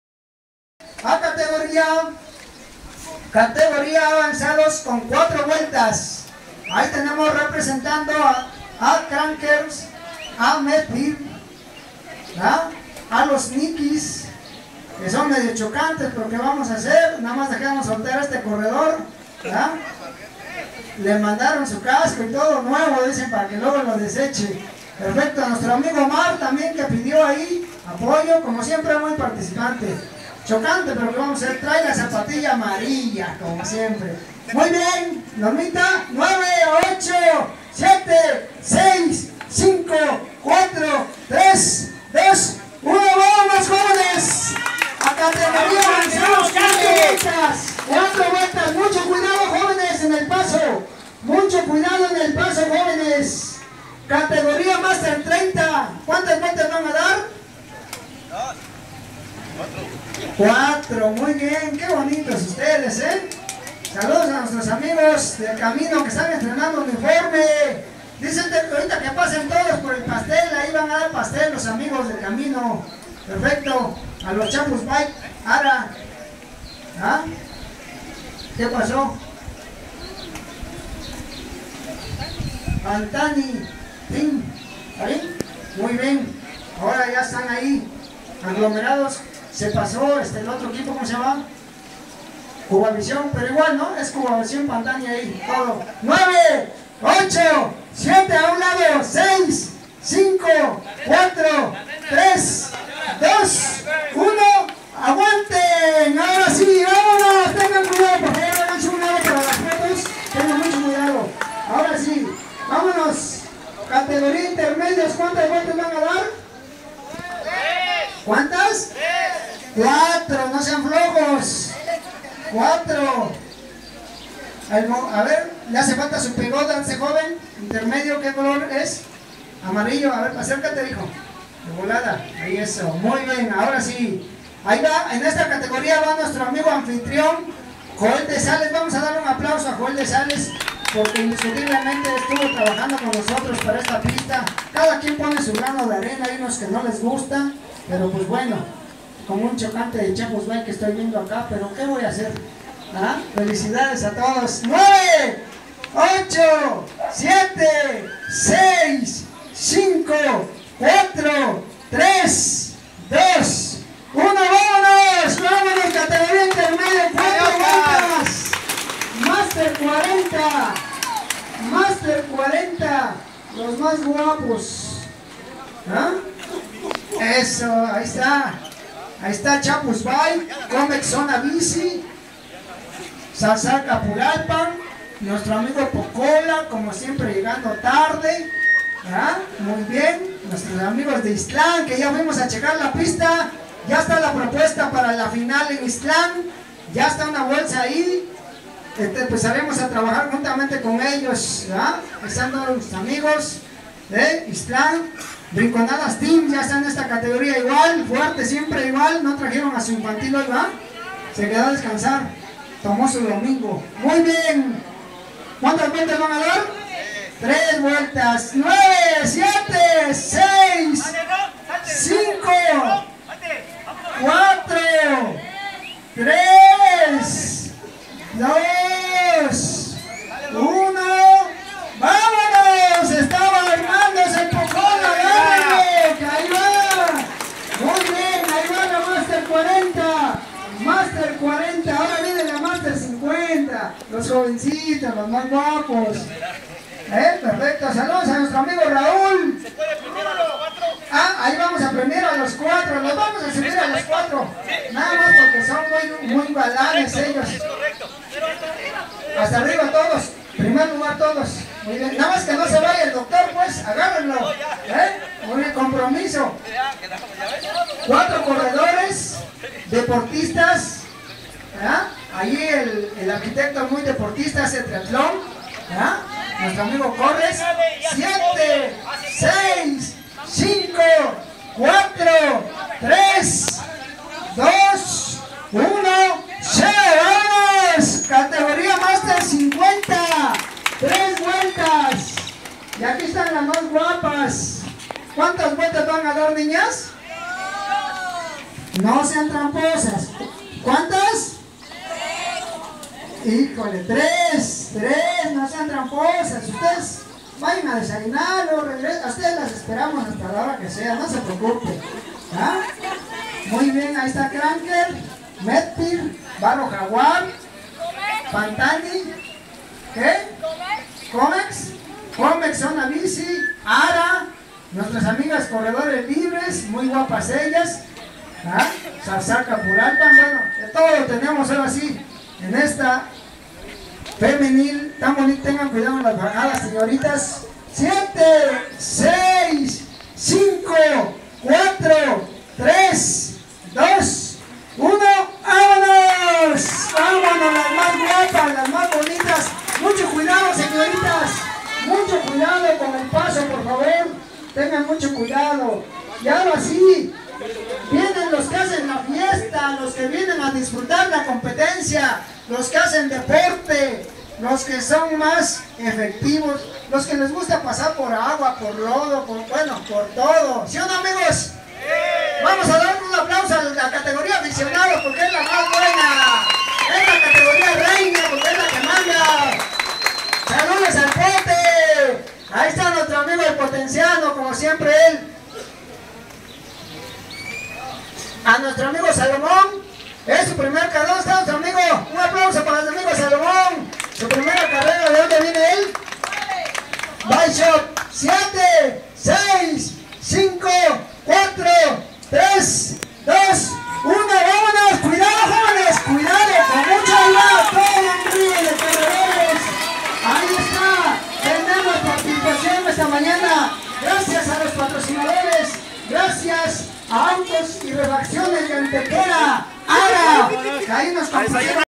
Sexta fecha de Mtb. Sn. Pedro Ixtlahuaca,Oax.
Salida de corredores por categorías.